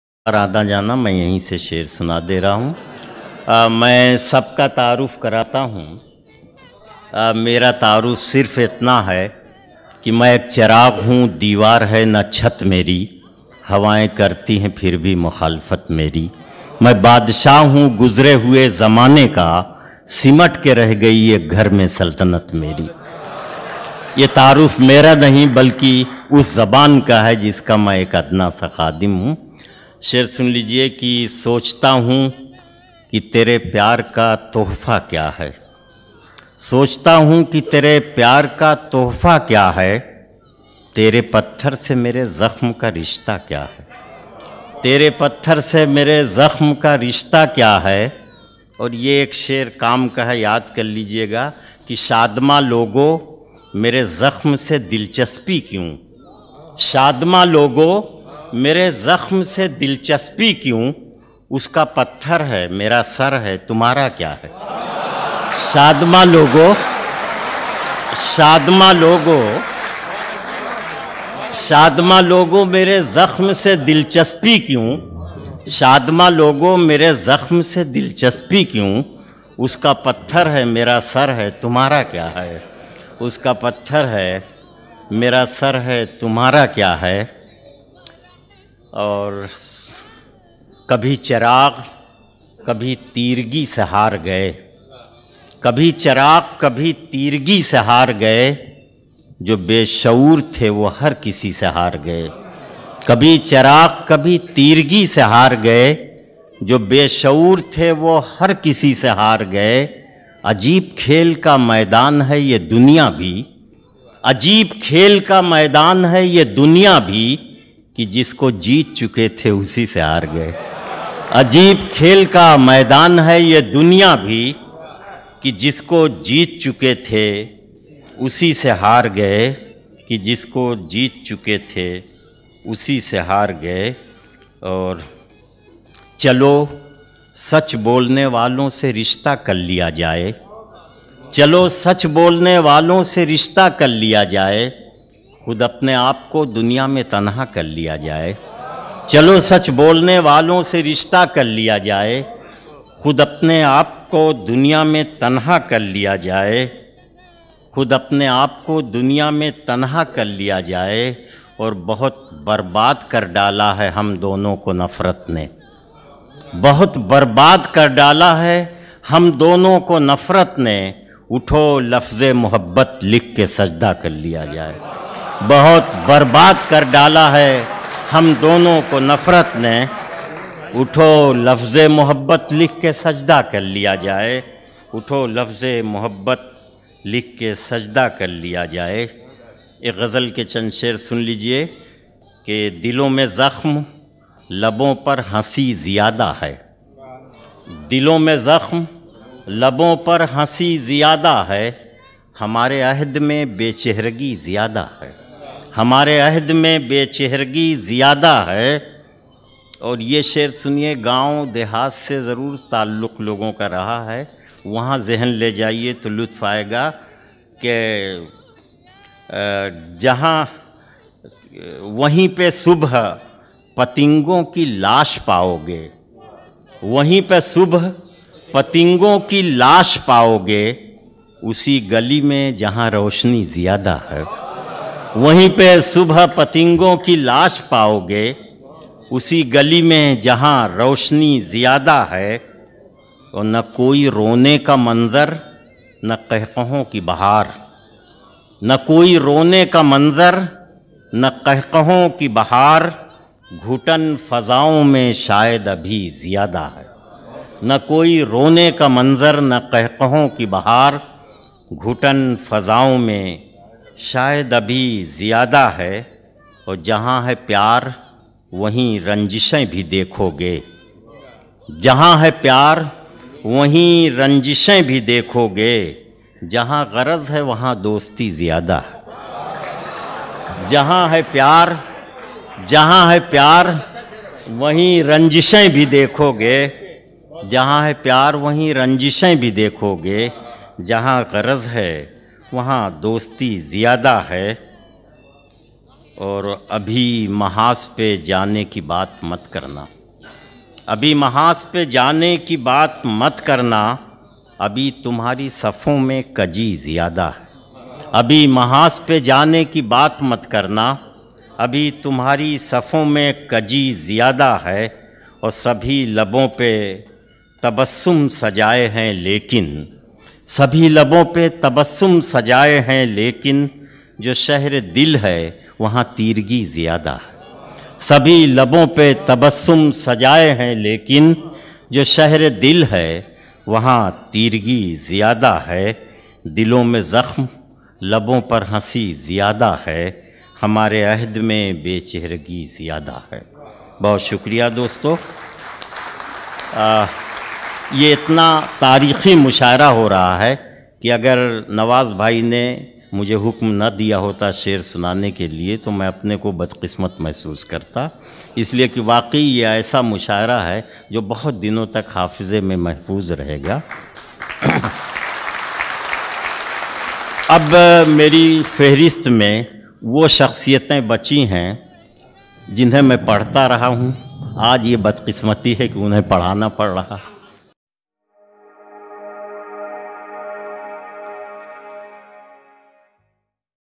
Anwar Jalalpuri Sahab ki Shayari || अनवर जलालपुरी साहब की शायरी
Mai charag hoon, na deewar hai na chhat meri,
Hawayen fir bhi karti hai Mukhalifat meri,
Mai badshah hoon gujare huye zamane ka,
Simat ke rah gyi ye ghar me saltanat meri.

मैं चराग हूँ, न दीवार है न छत मेरी,
हवाएं फिर भी करती हैं मुख़ालिफत मेरी,
मैं बादशाह हूँ गुज़रे हुए ज़माने का,
सिमट के रह गयी ये घर मे सल्तनत मेरी।

For full content please see the Video...

Please write your comments in comment box...👇✍